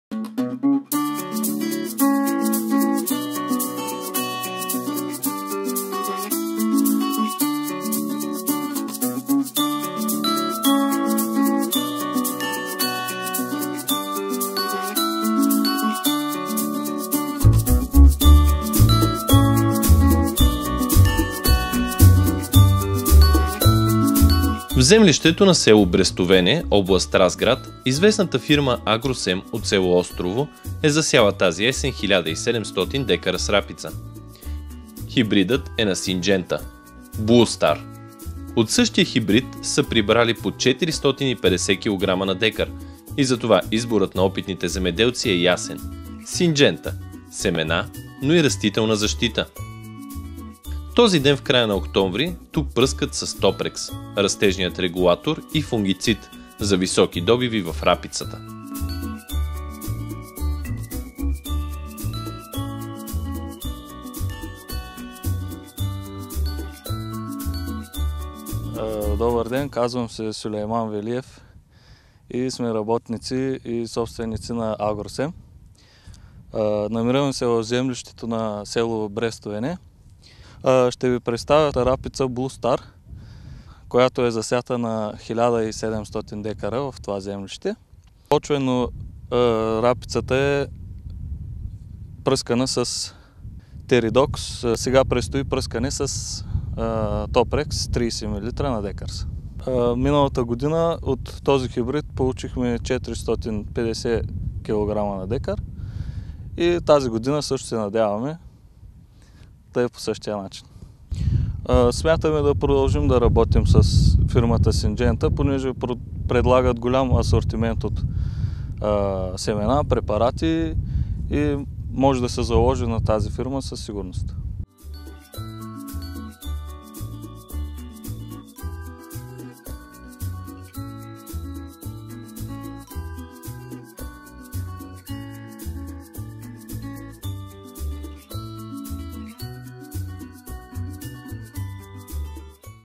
Есенни грижи за рапицата от Синджента
Топрекс и Теридокс - Ефективна растителна защита в рапицата от Синджента.